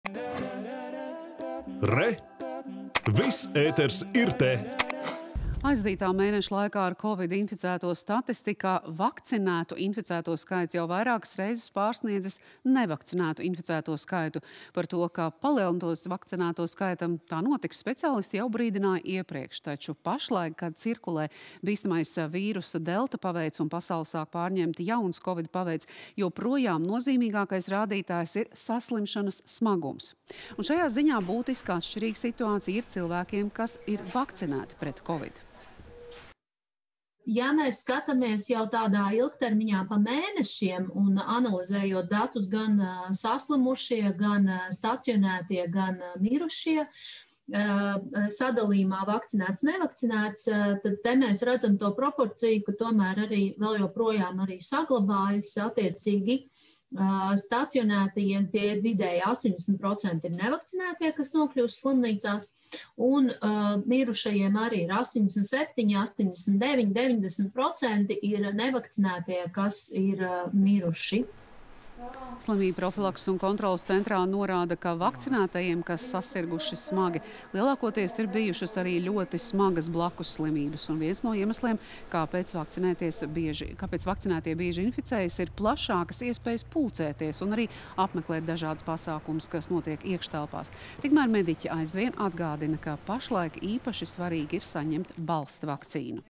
Arī vakcinētie bieži inficējas ar kovidu
Aizvadītā mēneša laikā ar kovidu inficēto statistikā vakcinētu inficēto skaits jau vairākas reizes pārsniedzis nevakcinētu inficēto skaitu. Par to, ka, palielinoties vakcinēto skaitam, tā notiks, speciālisti jau iepriekš tika brīdinājuši. Taču pašlaik, kad cirkulē bīstamais vīrusa delta paveids un pasauli sāk pārņemt jauns kovida paveids, joprojām nozīmīgākais rādītājs ir saslimšanas smagums.